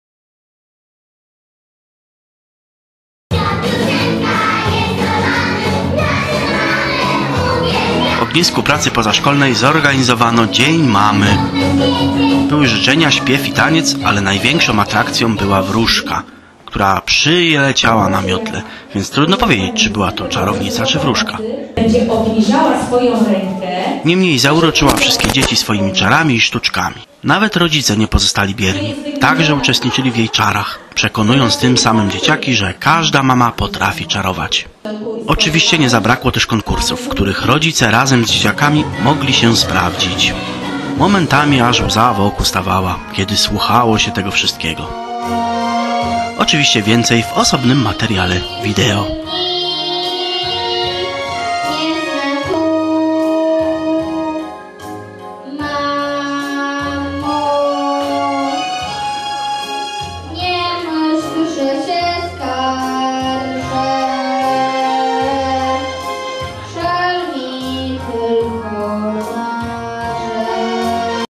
Dzień Mamy w Ognisku Pracy Pozaszkolnej
W Ognisku Pracy Pozaszkolnej zorganizowano Dzień Mamy. Były życzenia śpiew i taniec, ale największą atrakcją była wróżka, która przyleciała na miotle, więc trudno powiedzieć czy była czarownicą czy wróżką. Niemniej zauroczyła wszystkie dzieci swoimi czarami i sztuczkami.

Nawet rodzice nie pozostali bierni, także uczestniczyli w jej czarach. przekonując tym samym dzieciaki, że każda mam potrafi czarować.

Oczywiście nie zabrakło też konkursów:) w których rodzice razem z dzieciakami mogli się sprawdzić.

Momentami aż łza w oku stawała, kiedy słuchało się tego wszystkiego.

Oczywiście więcej w osobnym materiale video.